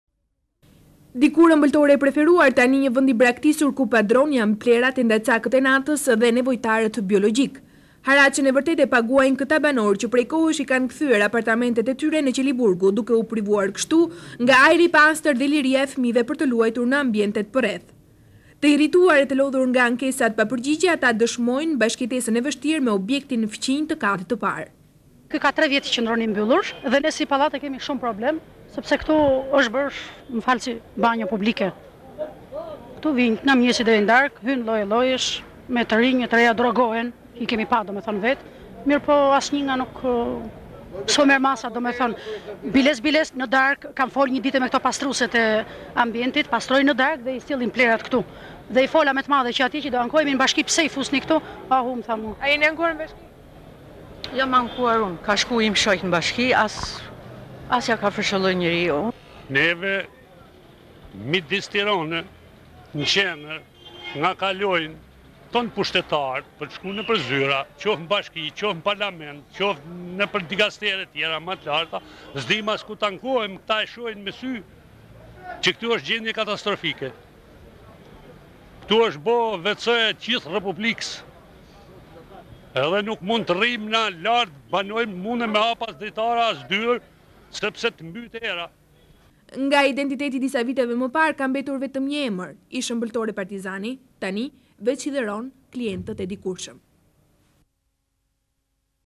Dikur "Ëmbeltore Partizani", sot banjo publike (20 Mars 1999)
Arkiva - Tv Klan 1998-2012; Pamje nga Embeltore Partizani,  kati i parë i pallatit i mbushur me plehra. Flasin banorët e pallatit.

Dikur ëmbëltore e preferuar tani një vend i braktisur ku padronë janë plehrat, endacakët e natës dhe nevojtarët biologjikë. Haraçin e vërtetë e paguajnë banorët të cilët janë të privuar nga ajri i pastër dhe liria e fëmijëve për të luajtur në ambjentet përreth. Të irrituar e të lodhur nga ankesat pa përgjigje ata dëshmojnë bashkëjetesën e vështirë me objektin fqinj të katit të parë.
Banorja: Ky ka tre vjet që qëndron i mbyllur dhe ne si pallat e kemi shumë problem sepse këtu është bërë si banjo publike. Këtu vijnë nga mëngjesi deri në darkë, hyjnë lloje llojësh, me të rinj e të reja, drogohen, i kemi parë vetë. Mirëpo asnjë s'po merr masa. Biles në darkë kam folur me këto pastrueset e ambjentit, pastrojnë në darkë dhe i sjellin plehrat këtu. I fola me të madhe që atje që do ankohemi në Bashki pse i fusni këtu, ohu tha.
Gazetarja: A jeni ankuar?
Banorja:  Jam ankuar unë. Ka shkuar im shoq në Bashki as ja ka fërshëllyer njeri.
Banori: Neve midis Tirane, në qendër nga kalojne të tanë pushtetarët për të shkuar nëpër zyra, qoftë në Bashki, qoftë në Parlament, qoftë nëpër dikastere të tjera më të larta, s'dimë as ku të ankohemi, ata e shohin me sy që këtu është gjendje katastrofike. Këtu është bërë WC e gjith Republikës. Dhe nuk mund të rrimë ne lart banojm, nuk mundemi me hap as dritare as dyer sepse të mbyt era.
Nga identititeti i disa viteve më parë, ka mbetur vetëm një emër, ish- Ëmbëltore Partizani, tani veç hidhëron klientët e dikurshëm.

Gazetare : Eni Vasili